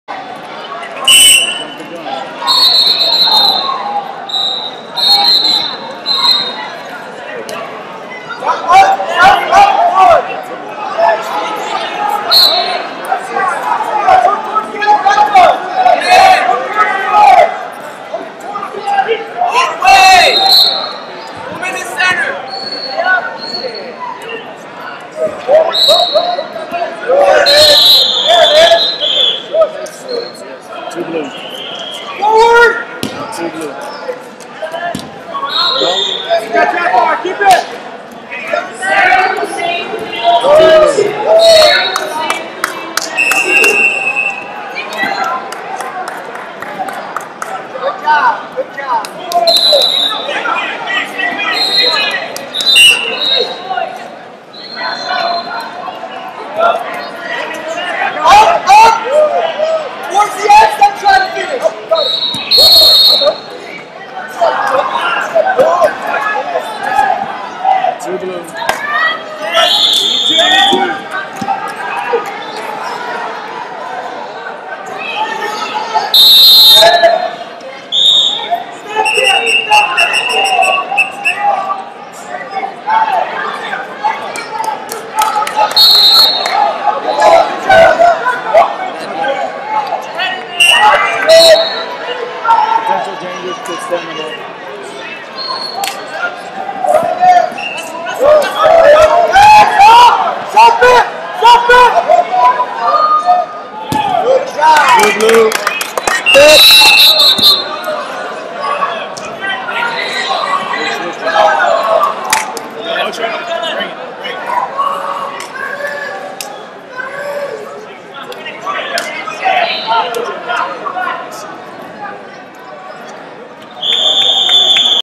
70 Kg Prelims - Jackson Dean, Pennsylvania RTC Vs Marcus Robinson, Cleveland Regional Training Cen
FloSports/2021 Northeast Regionals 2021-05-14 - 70 Kg Prelims - Jackson Dean, Pennsylvania RTC Vs Marcus Robinson, Cleveland Regional Training Center B80a